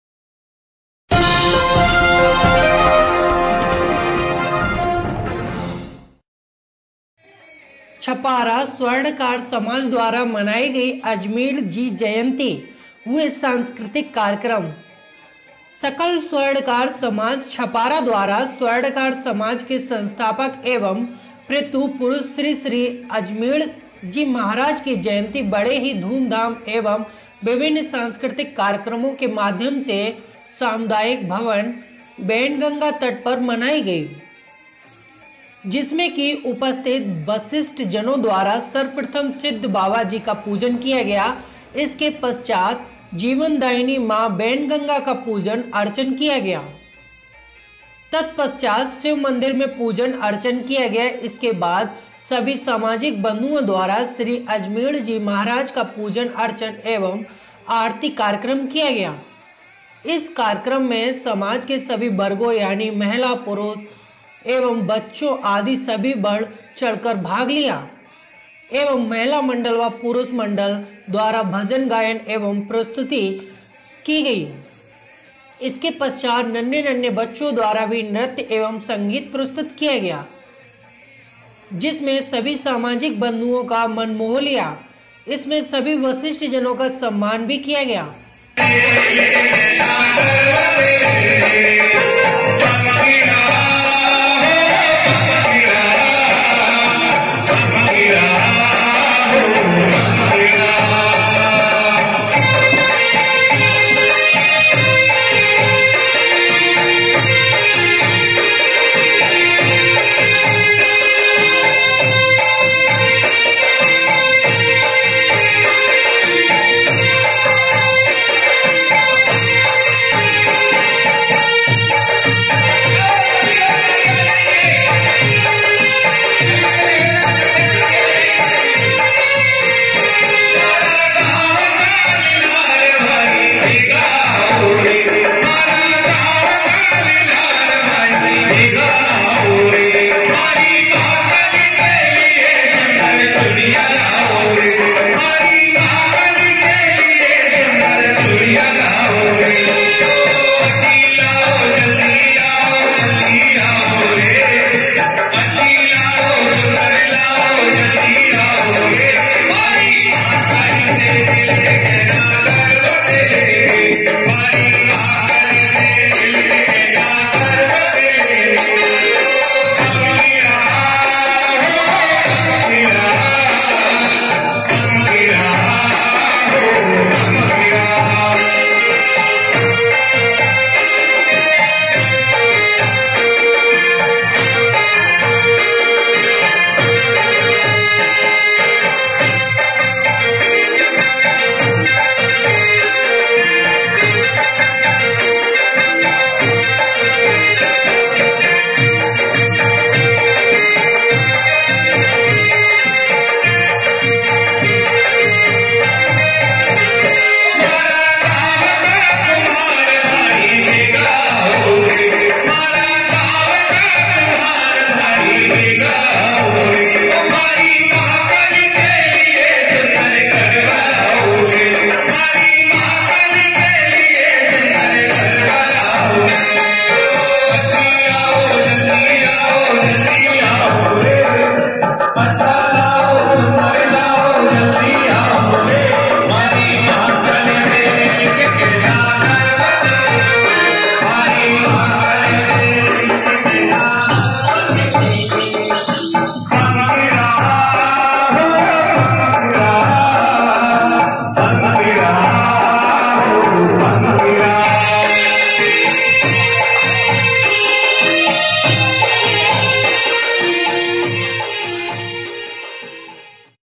छपारा स्वर्णकार समाज द्वारा मनाई गई अजमीढ़ जी जयंती हुए सांस्कृतिक कार्यक्रम
रिपोर्ट- अमित श्रीवास्तव

छपारा - सकल स्वर्णकार समाज छपारा द्वारा स्वर्णकार समाज के संस्थापक एवं पितृ पुरुष श्री श्री अजमीढ़ जी महाराज की जयंती बड़े ही धूमधाम एवं विभिन्न सांस्कृतिक कार्यक्रमों के माध्यम से सामुदायिक भवन बैनगंगा तट पर मनाई गई ।
जिसमें की उपस्थित वरिष्ठ जनो द्वारा सर्वप्रथम सिद्ध बाबा जी का पूजन किया गया , इसके पश्चात जीवनदायिनी मां बैनगंगा का पूजन अर्चन किया गया, तत्पश्चात शिव मंदिर मैं पूजन अर्चन किया गया इसके बाद सभी सामाजिक बंधुओं द्वारा श्री अजमीढ़ जी महाराज का पूजन अर्चन एवं आरती कार्यक्रम किया गया ।
इस कार्यक्रम में समाज के सभी वर्गों यानी महिला पुरुषों एवं बच्चों आदि सभी ने बढ़ चढ़कर भाग लिया एवं महिला मंडल व पुरुष मंडल द्वारा भजन गायन एवं स्तुति प्रस्तुत की गई इसके पश्चात नन्हें-नन्हें बच्चों द्वारा भी नृत्य एवं संगीत प्रस्तुत किया गया जिसमें सभी सामाजिक बंधुओं का मन मोह लिया इसमें सभी वरिष्ठ जनों का सम्मान भी किया गया एवं पर्यावरण संरक्षण हेतु वरिष्ठ जनों द्वारा उद्बोधन भी दिया गया । एवम स्वर्णकार समाज के इतिहास से नयी पीढ़ी को अवगत कराया गया ।
सभी सामाजिक बंधुओं के लिए भोजन आदि की व्यवस्था भी की गई थी इस कार्यक्रम को सफल बनाने में नवयुवक कार्यकारिणी मंडल स्वर्णकार समाज के युवकों का मुख्य योगदान रहा एवं सभी सामाजिक बंधुओं का विशेष सहयोग रहा ।

और अगर आप ने ग्रुप जॉइन नही किया है तो जॉइन करे